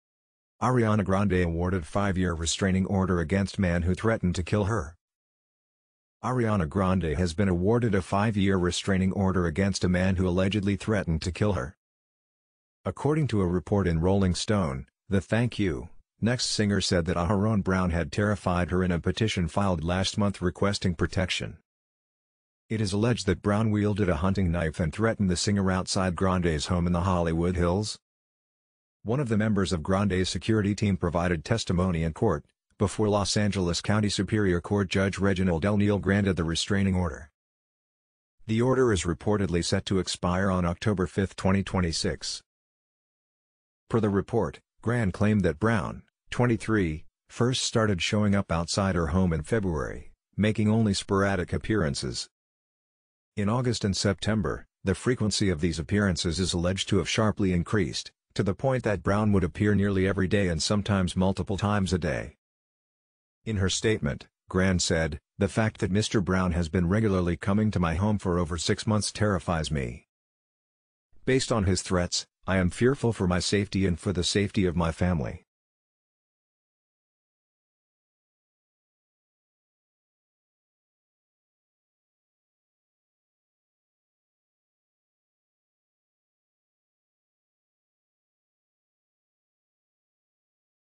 Ariana Grande wins five-year restraining order against man accused of threatening to kill her
Ariana Grande was granted a five-year restraining order against a man she accused of visiting her Los Angeles home multiple times, making death threats and wielding a large hunting knife.

On Tuesday, Los Angeles County Superior Court Judge Reginald Neal approved the protection order until 2026 for Grande, 28, whose real name is Ariana Grande-Butera, following testimony from one of the pop star's security guards.

In the order, filed in Los Angeles Superior Court on Sept. 14, Grande alleges that the man, Aharon Zebulun Brown, 23, terrorized her home for months, even though she had never given him her address.

Grande alleged that in his third and last visit to her residence on Sept. 10, Brown showed up "brandishing a knife and yelling threatening statements." When confronted by her security, Brown said, "I'll f---ing kill you and her," she alleged.